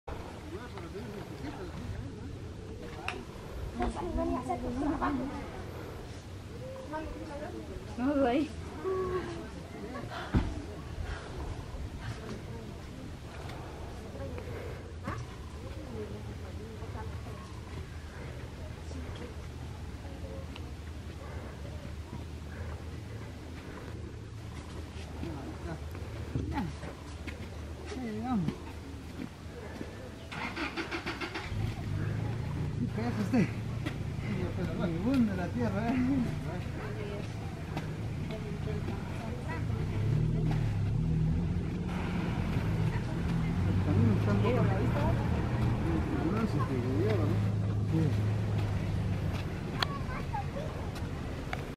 My time my life my love in Guatemala
How difficult to get these stoves up and down the steep terrain.